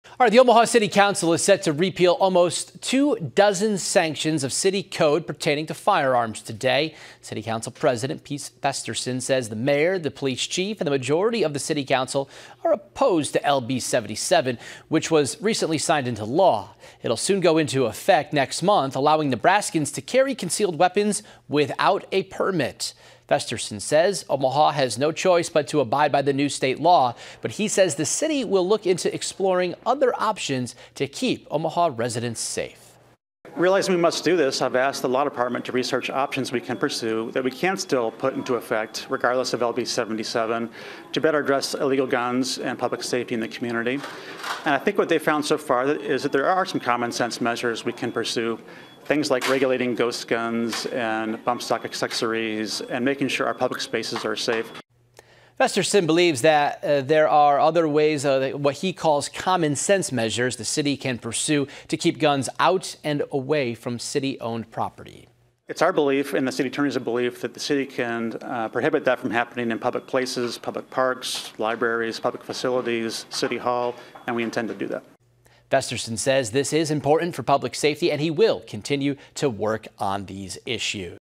Omaha City Council set to eliminate gun ordinances ahead of LB77 taking effect
The Omaha City Council plans to cancel gun ordinances as Nebraska LB77 takes effect Saturday.